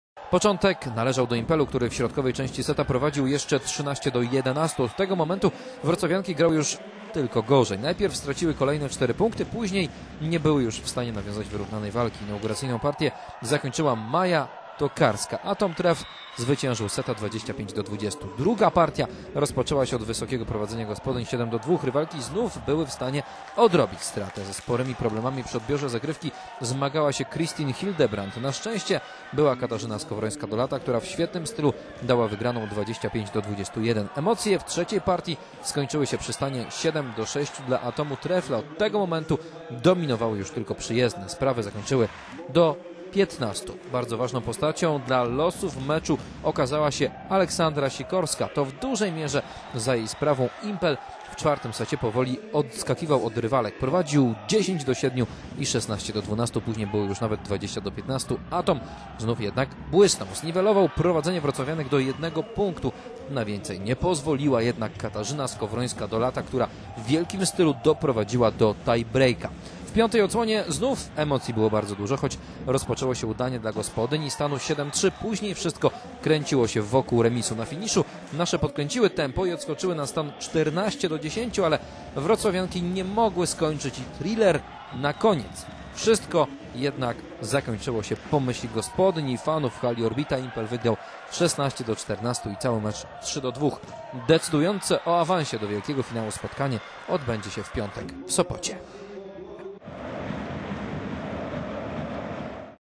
Impel wygrał i wciąż walczy o finał
Wszystko rozstrzygnie się w Trójmieście. W drugim spotkaniu półfinałowym Orlen Ligi Impel Wrocław pokonał PGE Atom Trefl Sopot 3:2. W rywalizacji, która toczy się do dwóch zwycięstw, jest remis 1:1. Decydujące spotkanie w piątek w Sopocie. Na zwycięzce tej pary w wielkim finale już czeka Chemik Police.